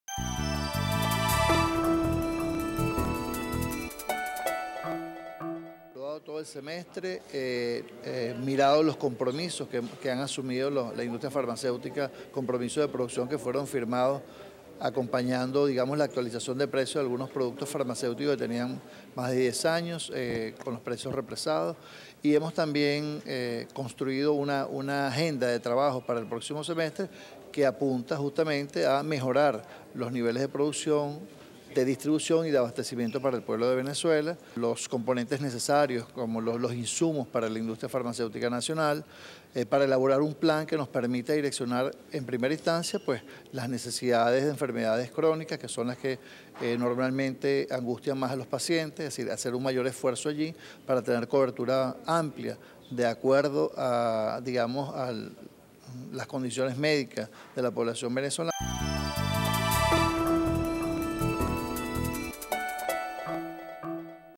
Pérez Abad acordó con sector farmacéutico actualizar precios y mejorar producción
El vicepresidente para el Área Económica, Miguel Pérez Abad sostuvo una reunión con el sector salud en la que se evaluó la situación de las empresas farmacéuticas  y como parte de los acuerdos se contempla realizar la actualización de precios de los fármacos así como la mejora en cuanto a producción además de abastecimiento.